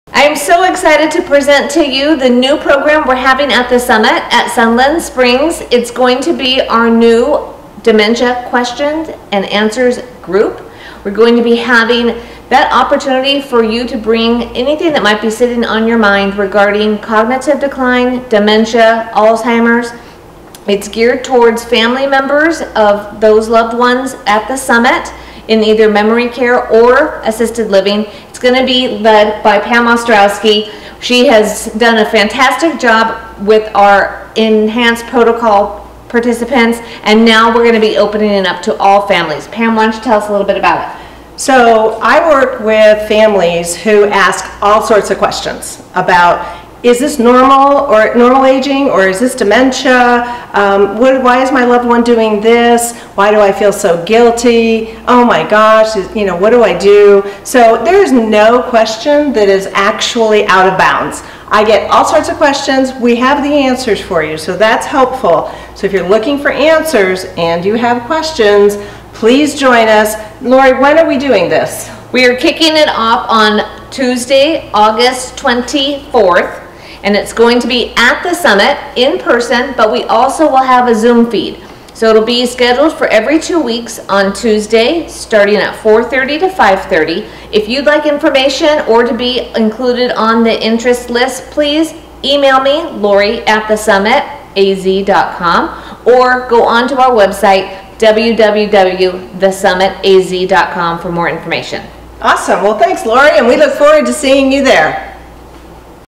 Dementia Q & A Support Group at The Summit at Sunland Springs
Announcing the launch of the Dementia Q & A Support Group, Part Support, Part Insight, All Real
Starting Tuesday, August 24, 2021, September 7, September 21, October 5, October 19
In Person at The Summit at Sunland Springs 2415 S. Signal Butte Road, Mesa, AZ 85209